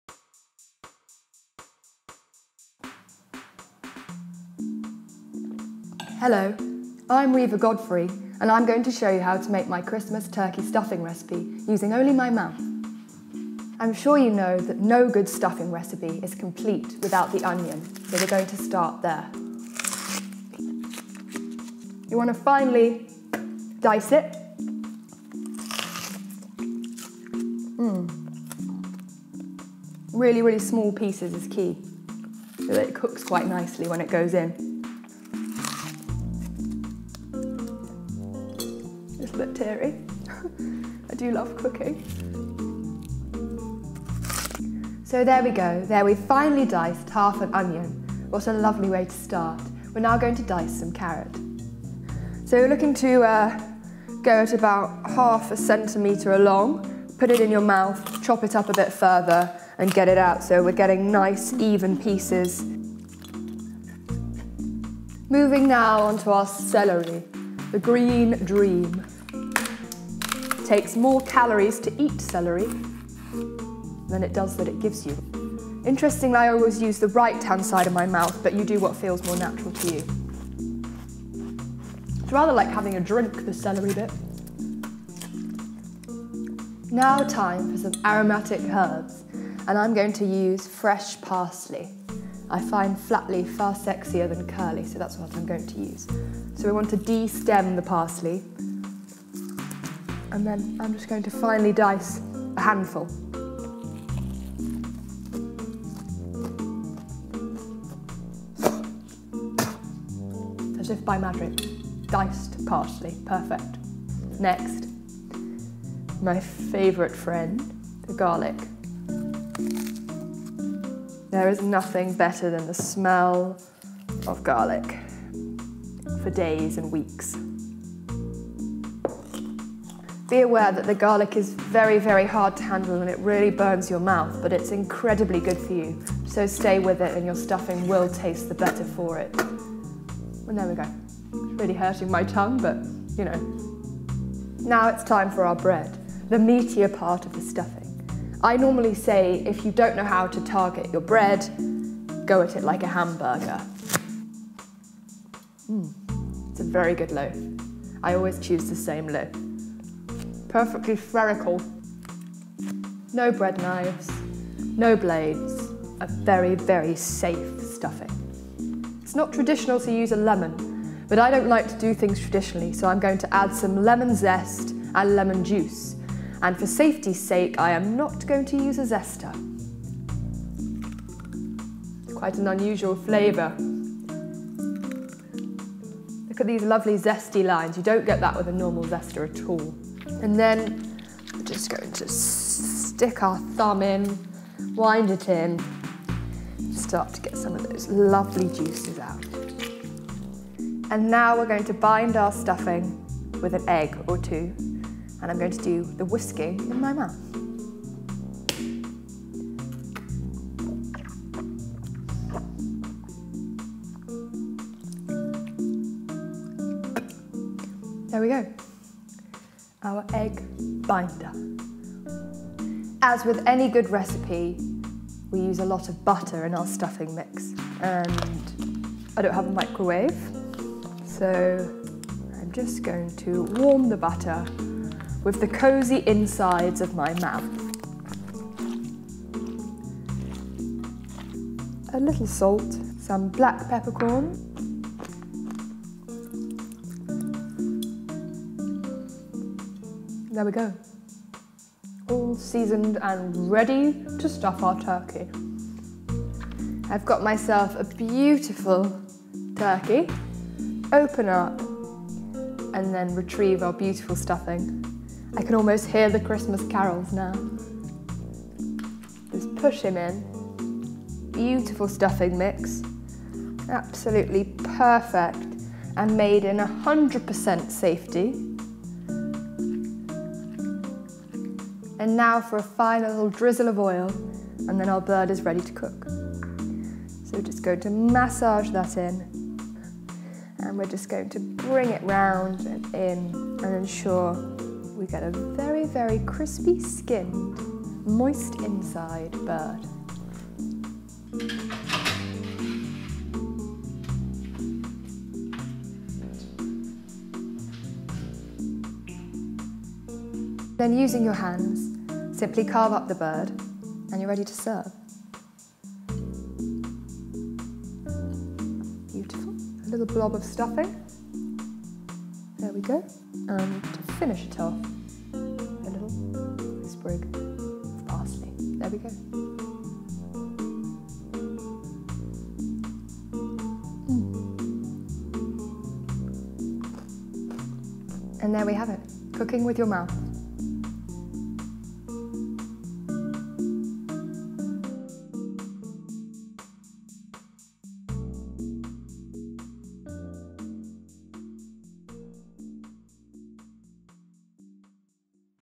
Cooking With Your Mouth | Christmas Turkey
How to make a Christmas Turkey Stuffing using the safety and comfort of your own mouth.